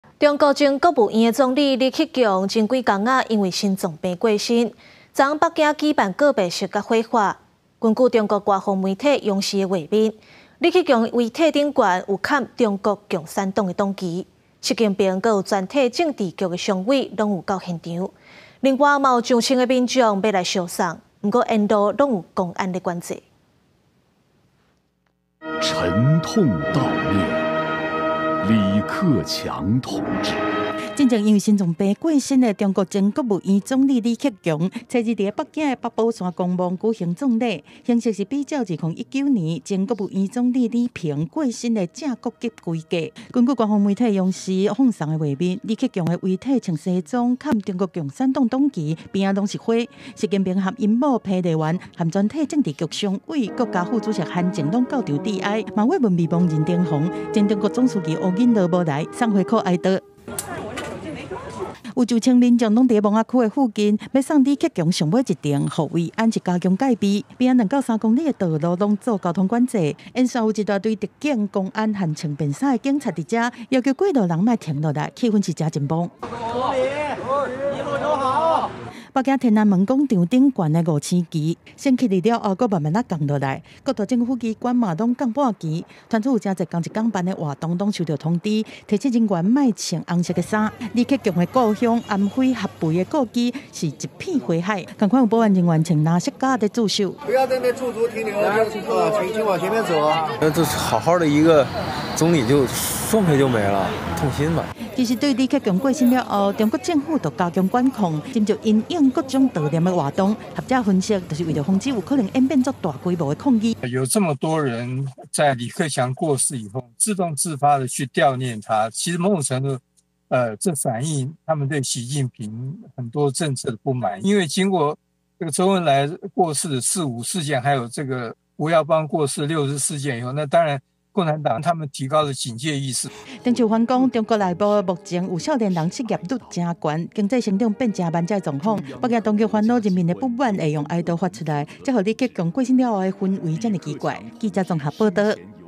李克強火化 習近平𤆬政治局常委到場致哀 | 公視台語台 | 20231103
中國前國務院總理-李克強、前幾工因為因心臟病過身，今仔日佇北京八寶山公墓、進行告別式佮火化。根據央視的畫面，李克強的遺體有蓋中共黨旗，習近平和全體政治局常委等人，攏有到現場悼念。另外也有上千名民眾咧公墓附近，欲來相送，沿路也攏有公安咧管制

四節新聞 ：　逐工（每天）09:00 / 12:00 / 15:00（拜一至拜五） / 19:00